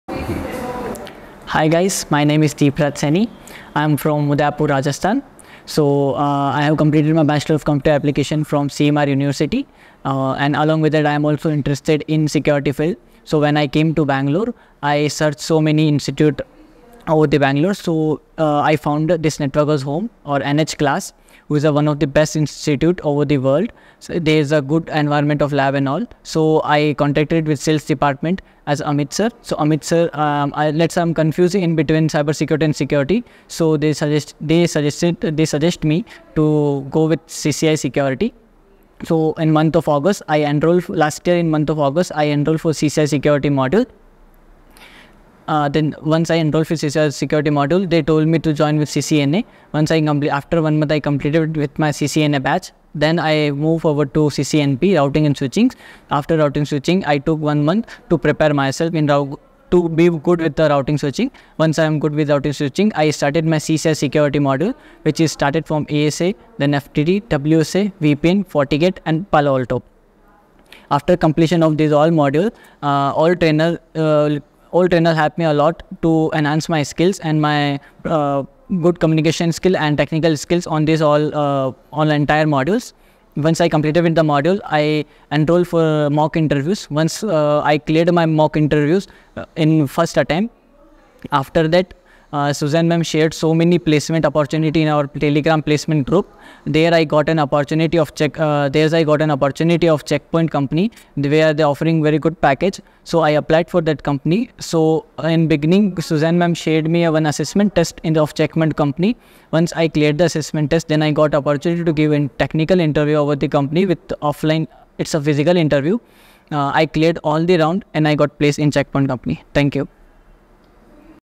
Placement Success Story of Deepraj Sainee | CCIE Training
This is the story of Deepraj Sainee,
"Hello everyone, I'm Deepraj Sainee, from Udaipur Rajasthan. I've completed my Bachelor's in Computer Application. I've enrolled in CCNA, CCNP, and CCIE Security programs at Networkers Home. I gained valuable insights into the Basics of Networking, Switching, Routing, and Firewalls like FTD, Checkpoint,  and Palo Alto. They provide excellent preparation classes and communication training, guided by experienced faculty members. Thanks to their training, I secured a position at CHECKPOINT. I highly recommend Networkers Home to anyone looking to build a career in networking. Thank you!."